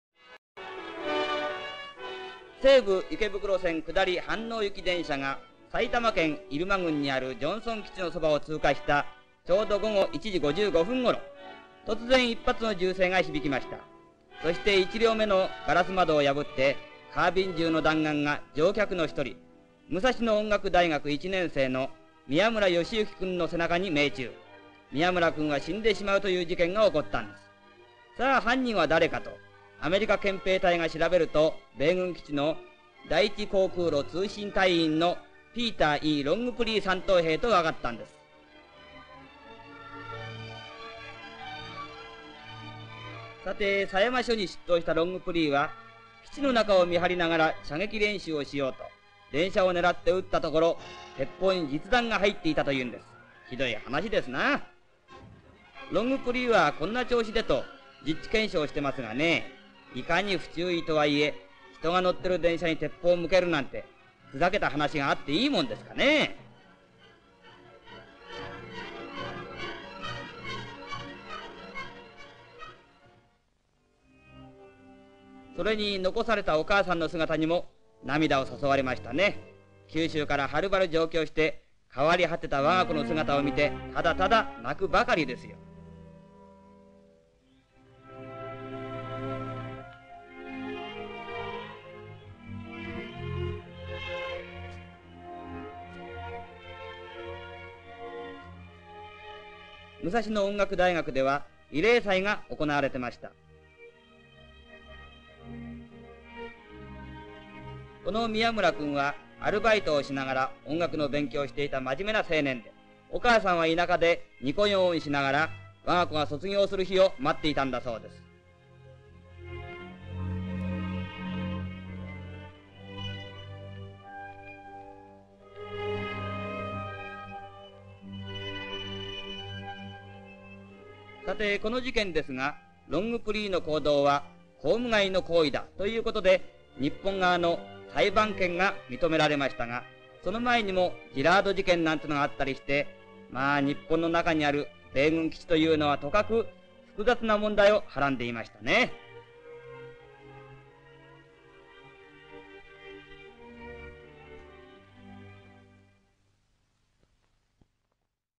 【TBSスパークル】1958年9月7日 米兵の発砲で学生死亡（昭和33年）
ご用命はTBSスパークル映像ライブラリーまで

9001_MKN-0907_01
1958年9月7日 米兵の発砲で学生死亡（昭和33年）

この映像は低画質版です。